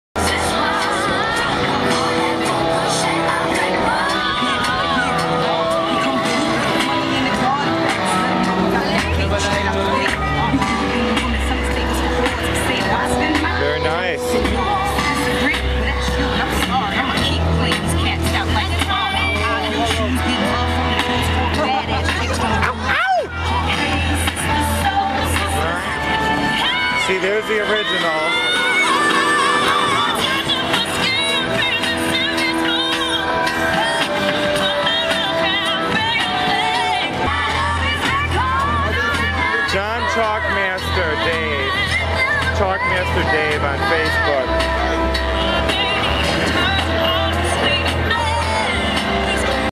Chalkmaster Dave on Yonge Street in Toronto
I saw Chalkmaster Dave this summer on Yonge Street doing a Megan Fox cover. What a character!